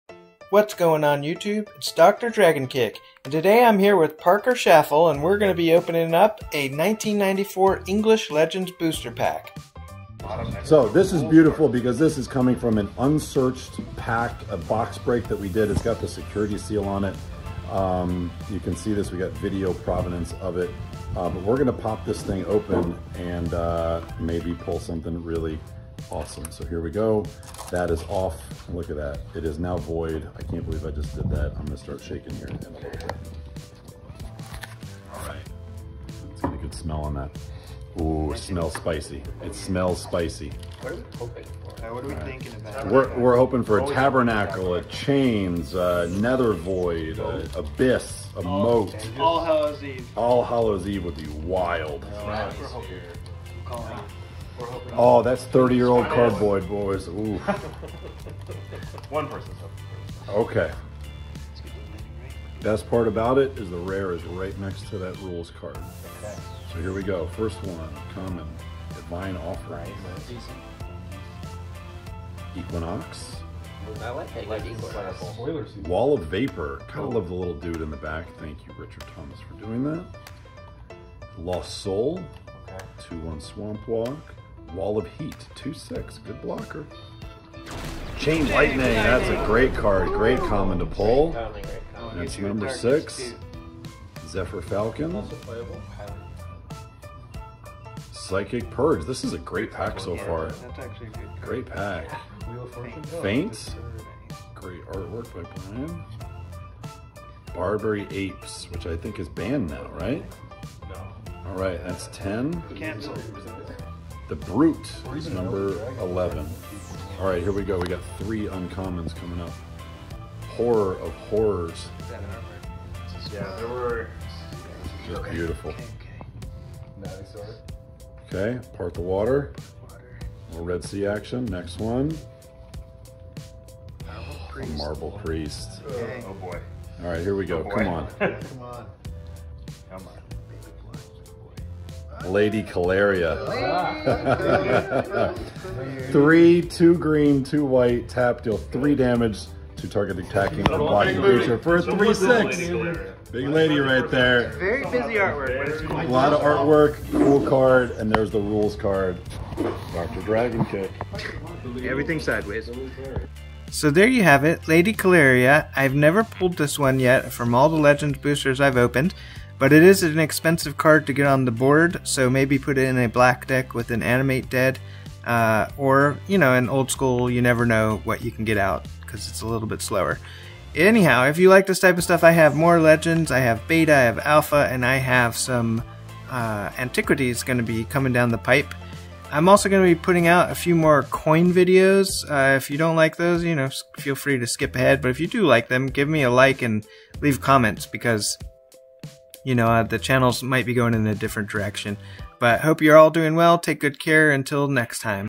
MTG Magic the Gathering English Legends 1994 Booster Opening (August 2023)
Opening a Magic the Gathering English Legends Booster pack from 1994!

August 2023